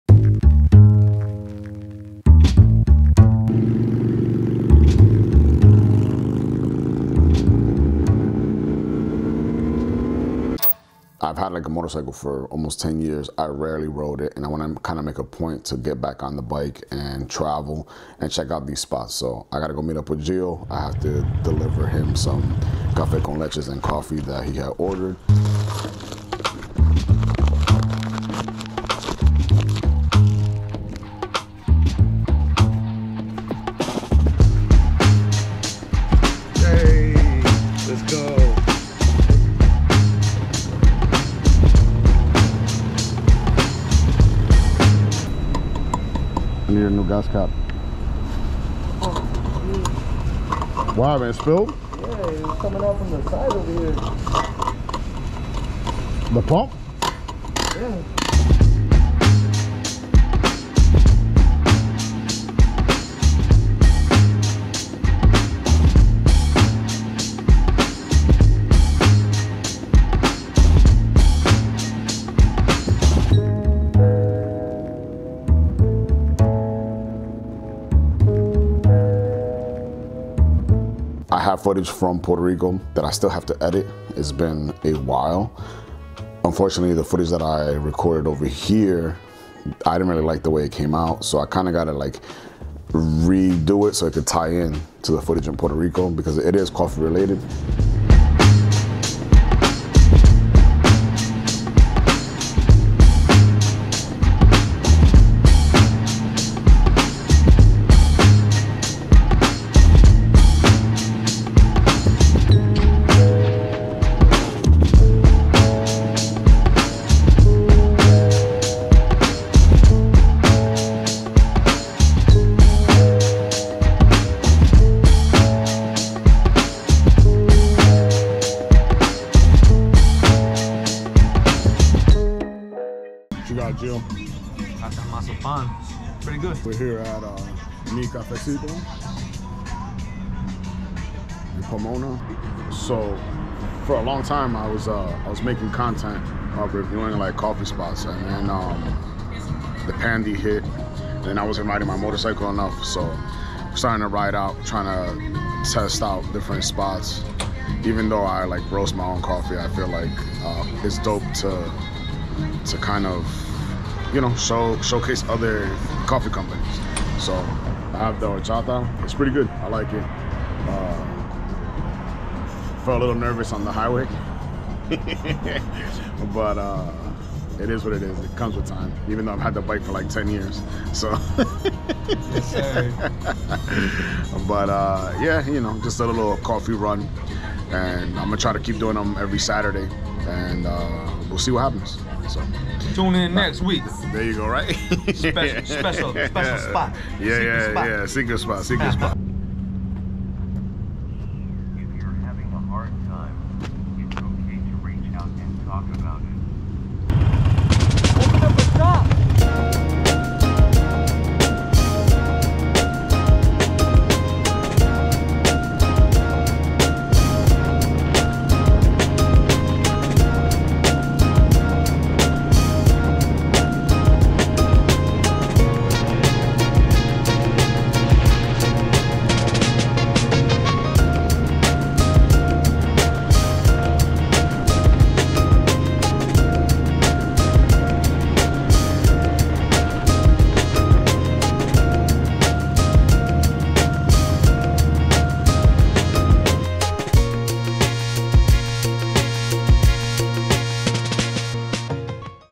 Coffee Run: Mi Cafecito
This is the second video where I ride out with friends to get coffee from local coffee shops. This time we visit Mi Cafecito out in Pomona, Ca. It's been some time that i have been on my motorcycle. This gives me an opportunity to venture out and get better at riding. Join me as I grow this part of my channel with a weekly goal of posting content from these other shops.